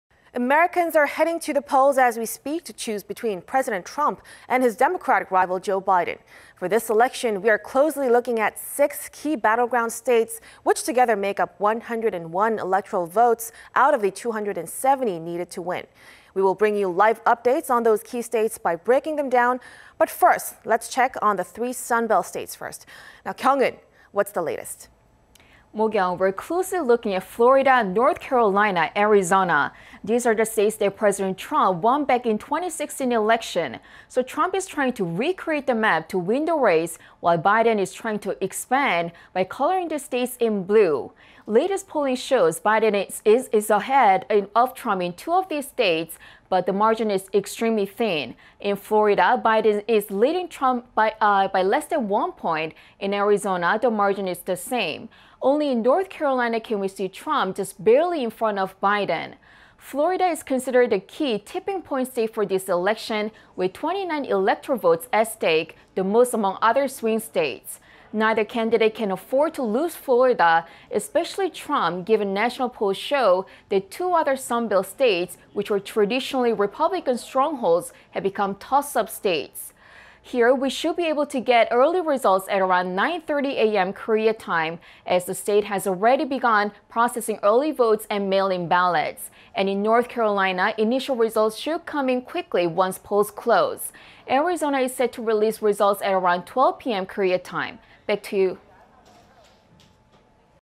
In person-voting in full swing for 2020 U.S. Presidential election; record 101 million voted early
미국 대선 당일 현장 투표 시작... 사전 투표는 1억 돌파

Americans are heading to the polls as we speak to choose between President Trump and his Democratic rival Joe Biden.
For this election, we are closely looking at 6 key battleground states, which together make up one-hundred-one electoral votes out of the two-hundred-seventy needed to win.
We will bring you live updates on those key states by breaking them down, but first let's check on the three Sunbelt states.
Kyung-eun, what's the latest?
Mok-yeon, we are closely looking at Florida, North Carolina, Arizona.
These are the states that President Trump won back in the 2016 election.
So Trump is trying to recreate the map to win the race,...while Biden is trying to expand by coloring these states in BLUE.
Latest polling shows, Biden is ahead of Trump in two of these states, but the margin is extremely thin.
In Florida, Biden is leading Trump by less than one point.
In Arizona, the margin is the same.
Only in North Carolina, can we see Trump, just barely in front of Biden.
Florida is considered the key, tipping point state for this election, with 29 electoral votes at stake the most among other swing states.
Neither candidate can afford to lose Florida,... especially Trump, given national polls show the two other Sunbelt states which were traditionally Republican strongholds have become toss-up states.
Here, we should be getting early results around 9:30 AM Korea time, as the state has already started processing early votes and mail-in ballots.
And in North Carolina, initial results should come quickly once polls close.
Arizona is set to release results at around 12 PM Korea time.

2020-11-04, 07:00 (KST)